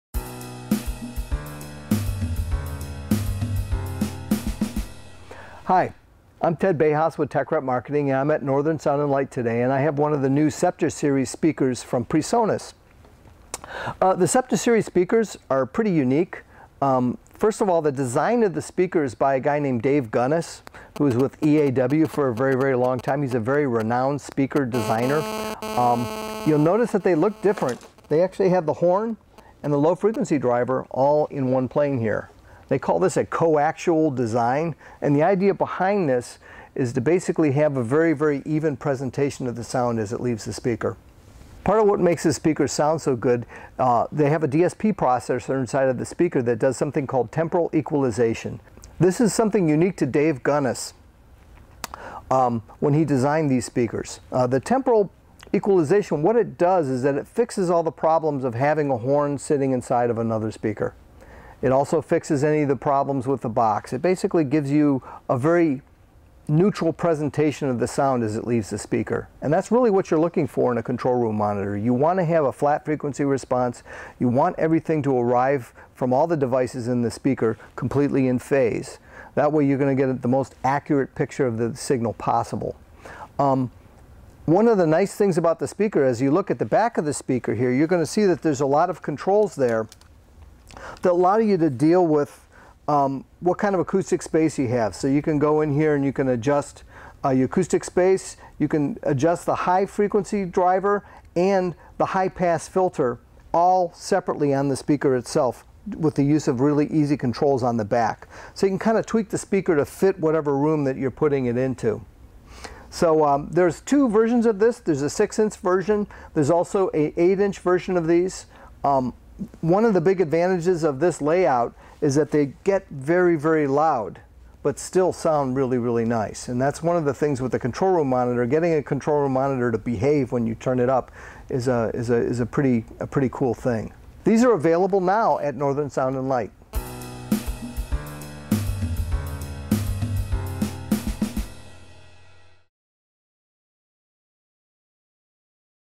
PreSonus Sceptre Studio Monitors | Northern Sound & Light
PreSonus Sceptre CoActual™ series studio reference monitors feature an advanced coaxial design that works integrally with a 32-bit, 96 kHz, dual-core processor that uses Fulcrum Acoustic's TQ™ Temporal Equalization technology. Spectre CoActual monitors deliver clarity and coherence that has previously only been available in ultra-high-end systems. Yet they are an affordable investment for project-studio owners—a first for DSP-controlled, TQ-based systems.